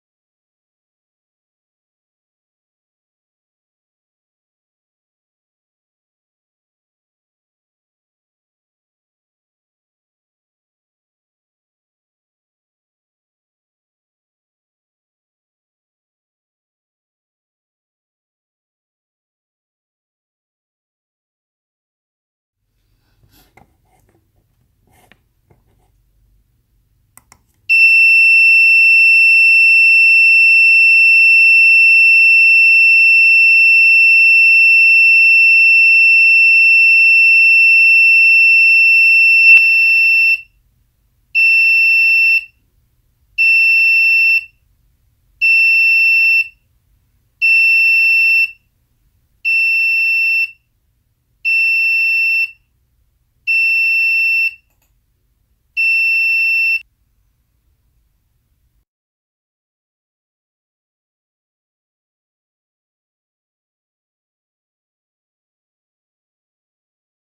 Test d'un buzzer piézoélectrique avec Scratch
Courte vidéo montrant le test d'un buzzer piézoélectrique connecté sur le GPIO 4 d'un Raspberry Pi. Programme utilisé : Scratch